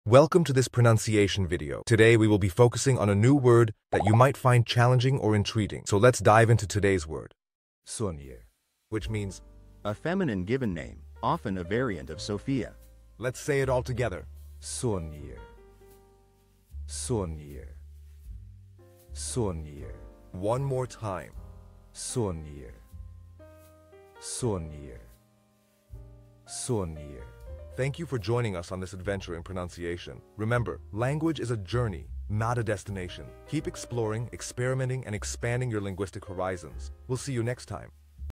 How to pronounce Sonya
Master the Pronunciation of 'Sonya WHICH MEANS SONYA' - which means : Female Given Name, often A Variant of Sophia. 📝 with @PortuguesePronunciation 🎓🗣️ - [Your Guide to Portuguese]

Learn to pronounce 'Sonya' perfectly in Portuguese with our easy and engaging tutorial by @PortuguesePronunciation! 📖📽️ In this comprehensive video, we provide you with native speaker examples 🗣️👂 and phonetic breakdowns 🎵 to ensure you get the pronunciation just right.

How to say Sonya in Portuguese